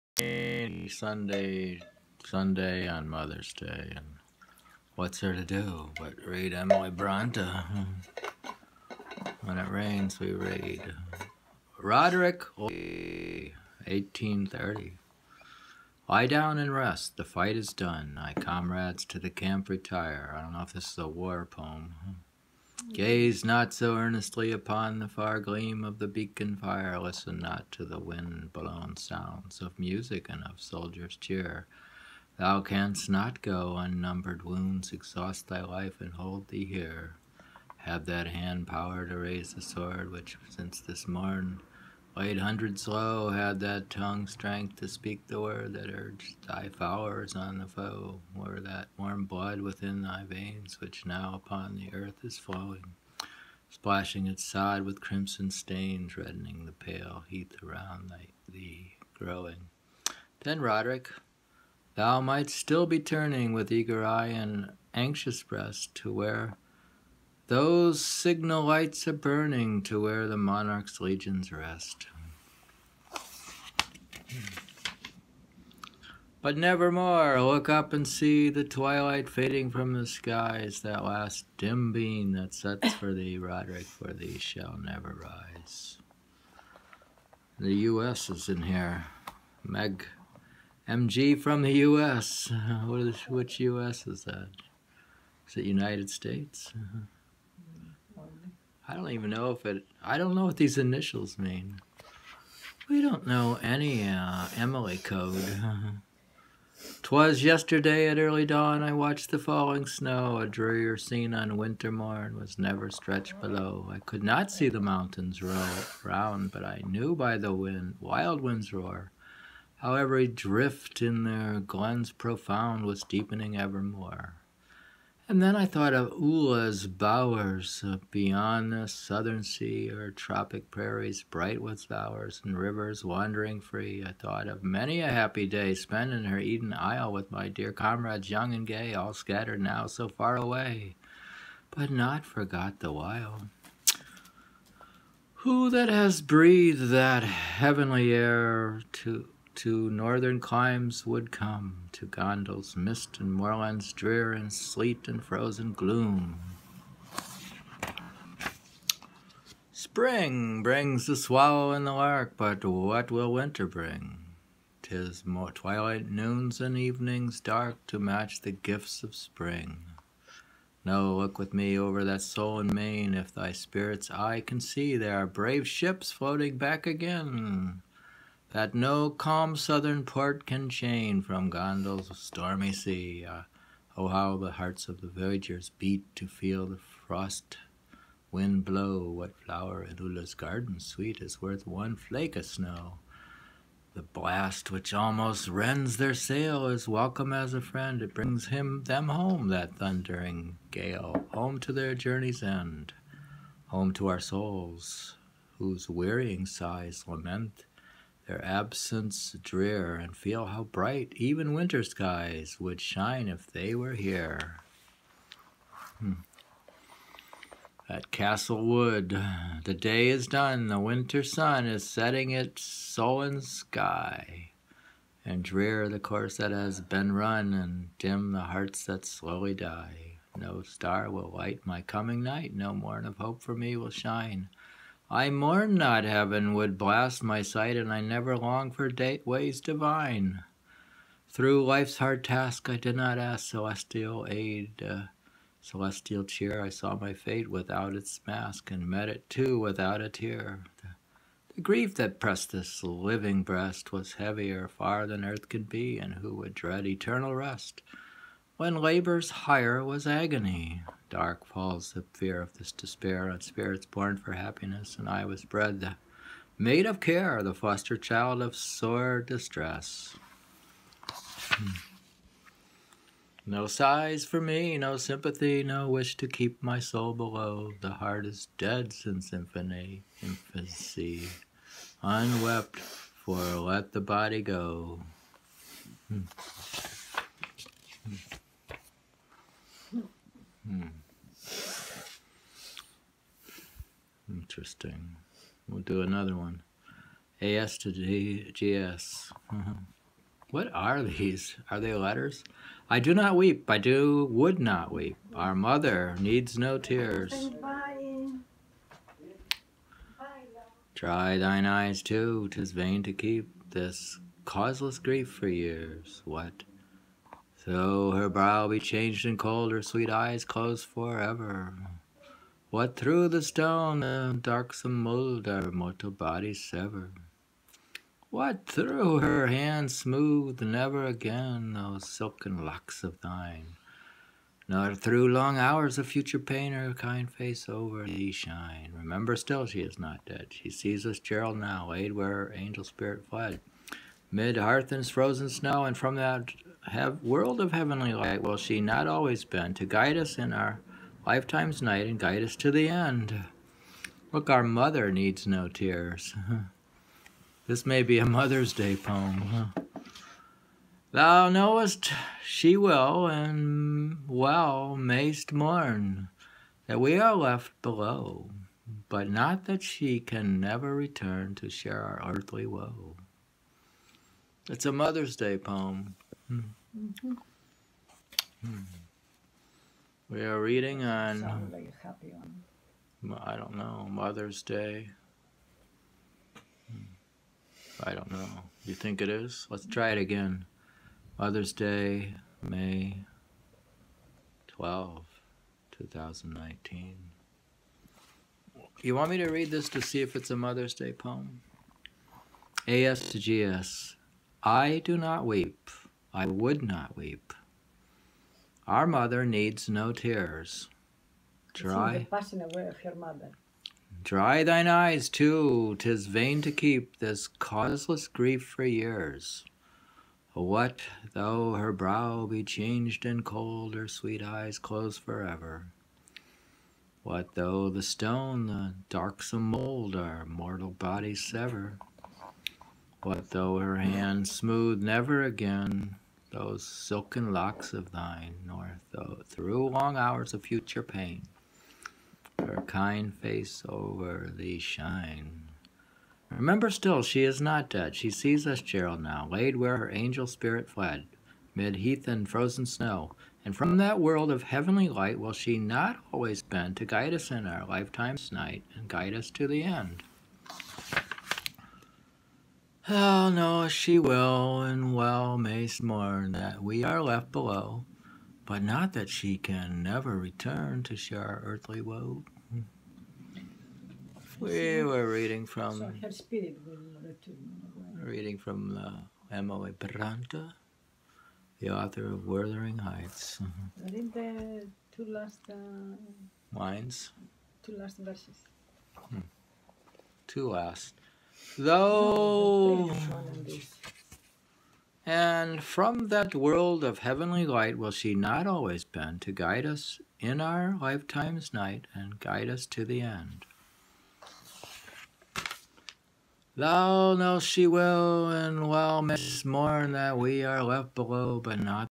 Emily Bronte Mother’s Day Poem
Emily Bronte Poetry reading I do not weep, I would not weep; Our Mother Everyman Library Pocket poetry read by David Smith